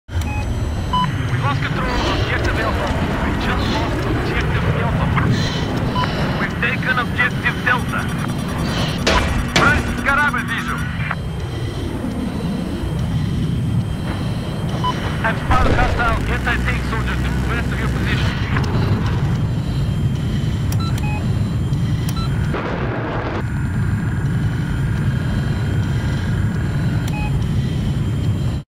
The delayed kill
I didn't think anyone was in that boat, It must have exploded after I flew by.  I had also just regained control of my jet after something hit me.